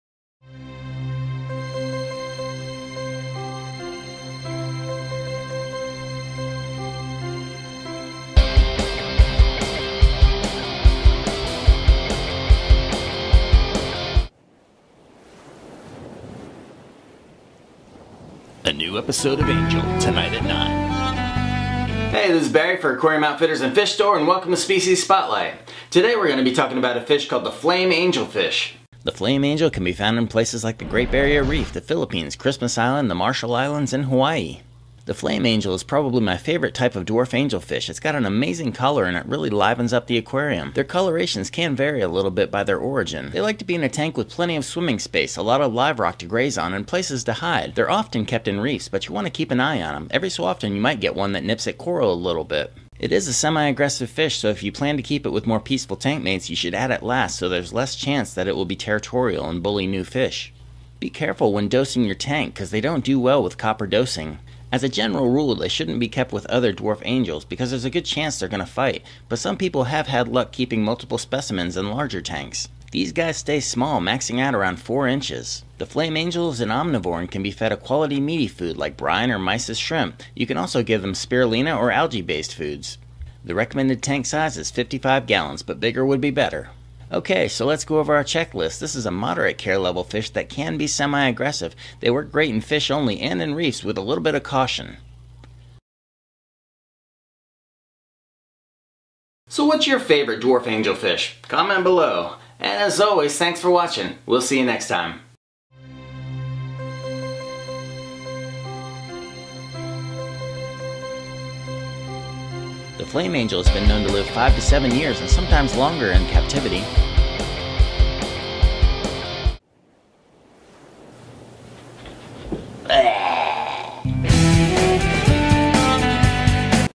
Species Spotlight Season 2 - The Flame Angel - Episode 22
Flame Angel: No doubt the most popular and widely considered the best looking of all the dwarf angels. Since this batch of Centropyge loricula are from the Marshall Islands the color patterns are far superior and much more unique than the standard Flame Angel. All Centropyge genus angels are hermaphroditic and this species has no distinguishable differences in color between male to female. Considered a reef caution species since some individuals may occasionally nip at corals and clams. The Flame Angel maxes out at 4 inches and requires at least a 25 gallon tank preferably with plenty of live rock to pick at. This is a peaceful easy care level reef-caution fish.